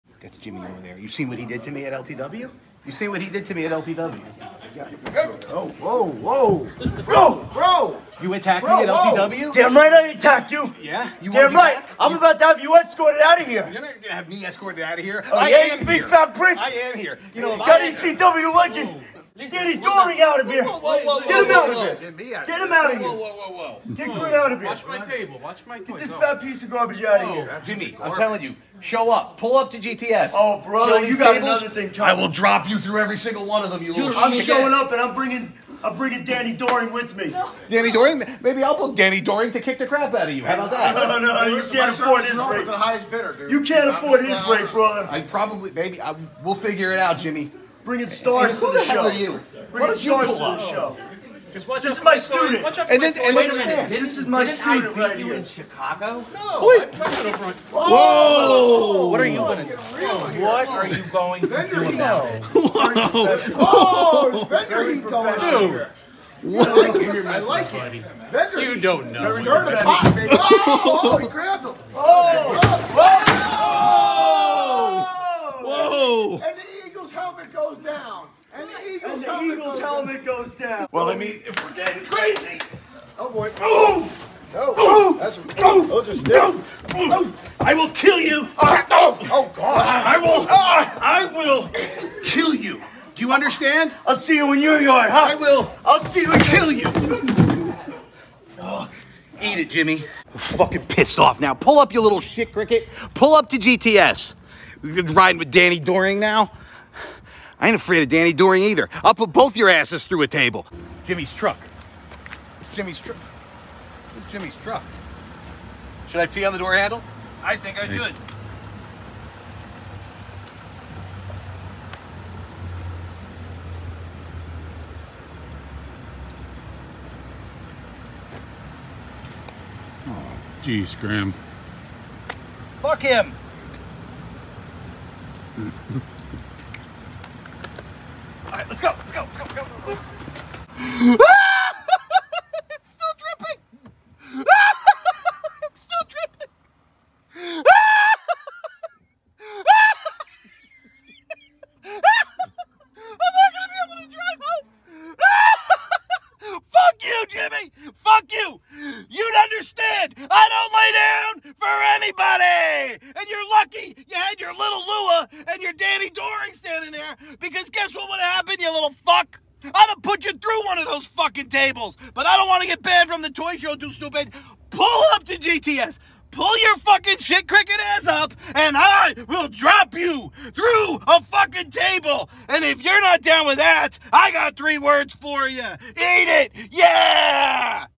GRIM vs JIMMY AT TOY SHOW!
GTS wrestling YouTube championship backyard wrestling main event parody of wwe survivor series and A table gets flipped at the toy show when Grimm confront￼s Jimmy controversy